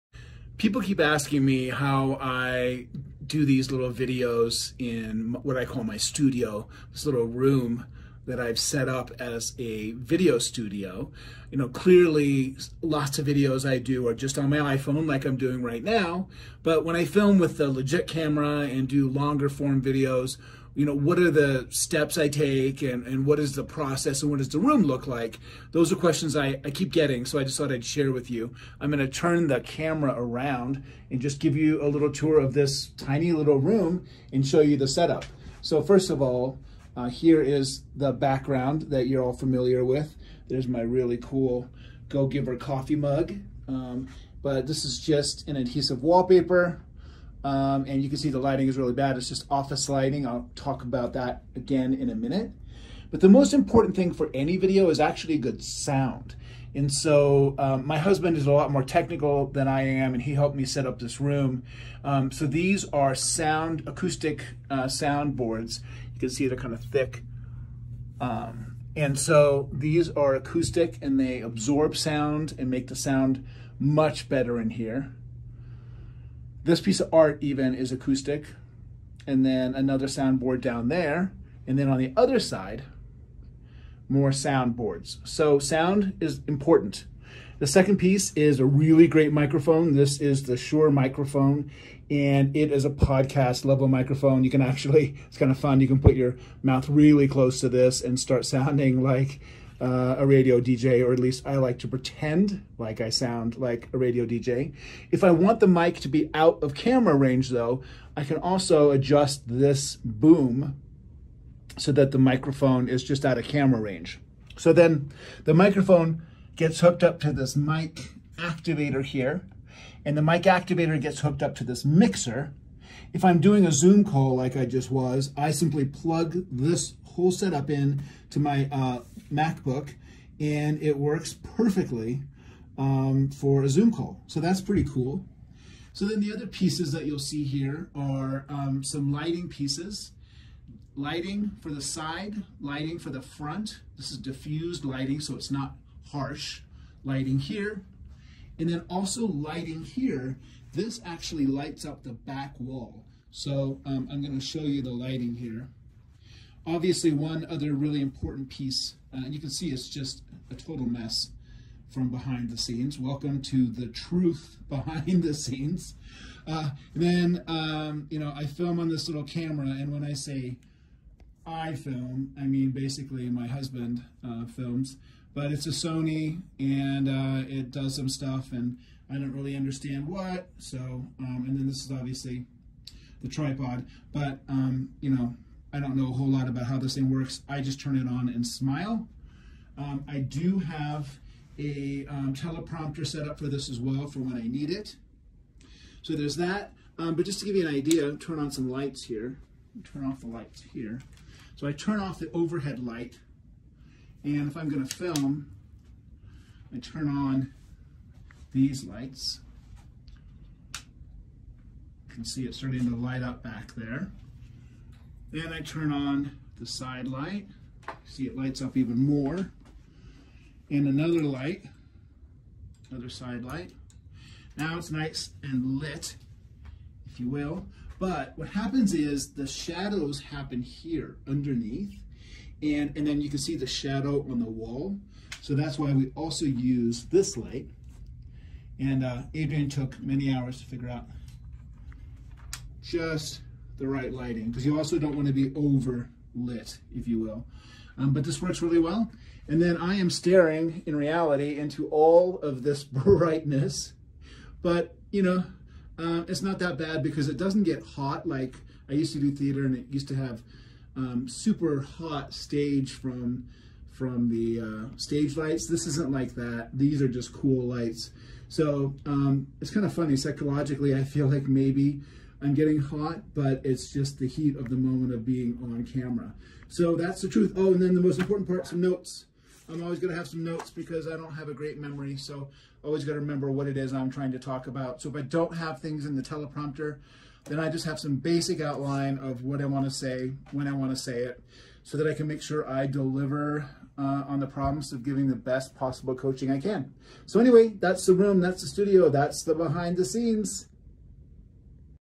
Behind The Scenes: Video Studio
🎥 Do you know what the most important piece of equipment to have in a video studio is? Take a guess in the comments 👇 and then watch the video to find out!